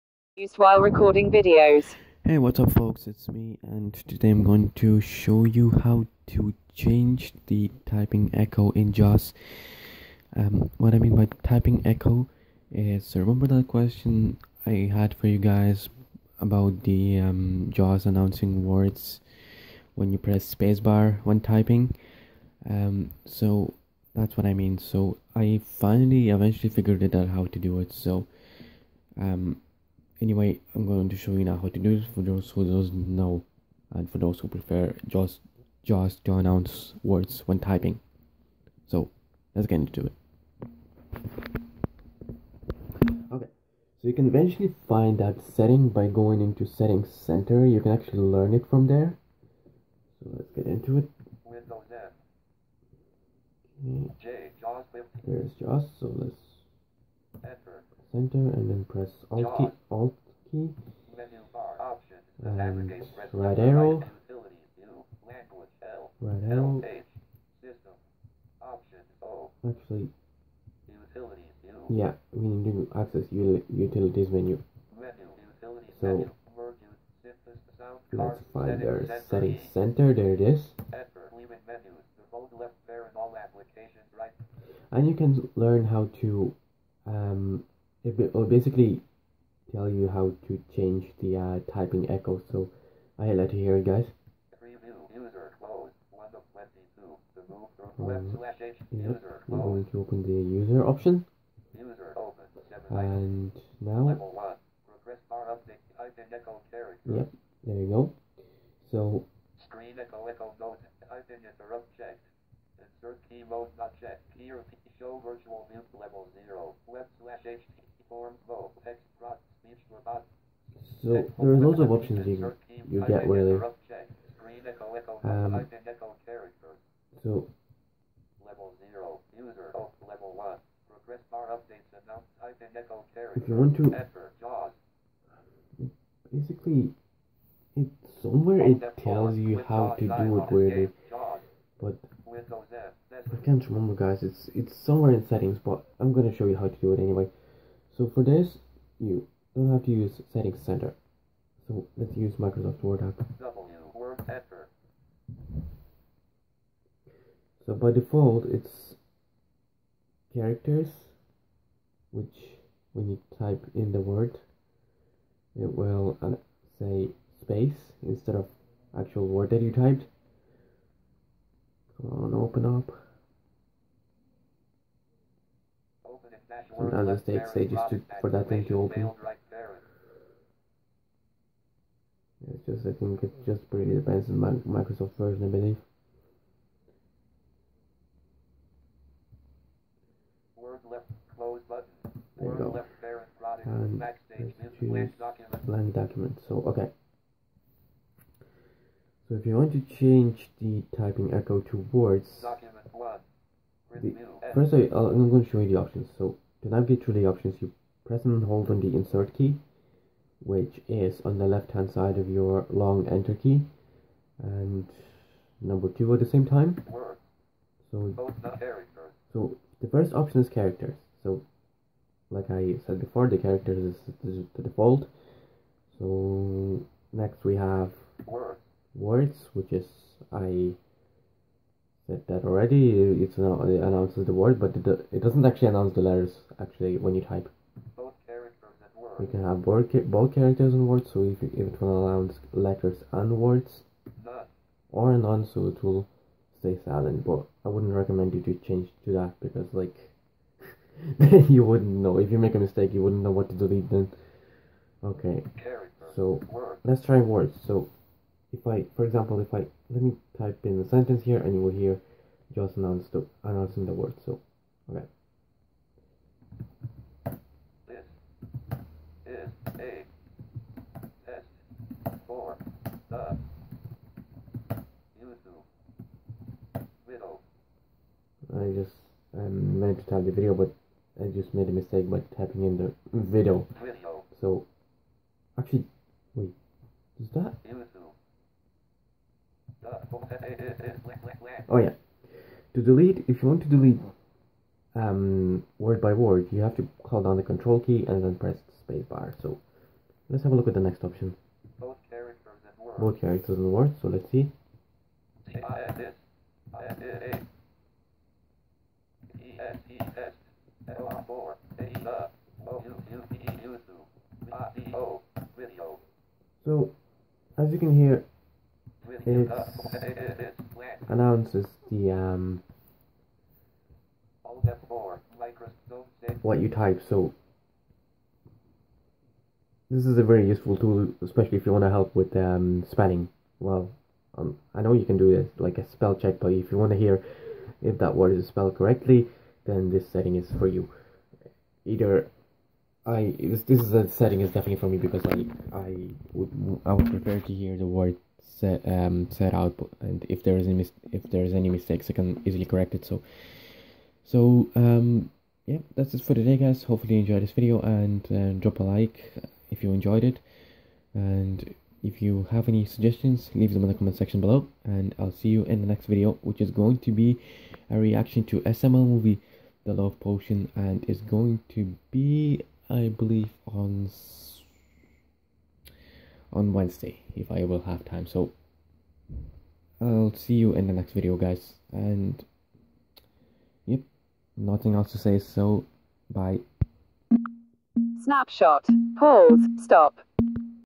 How to change typing Echo in Jaws screen reader
I show  how to change typing Echo settings in the screen reader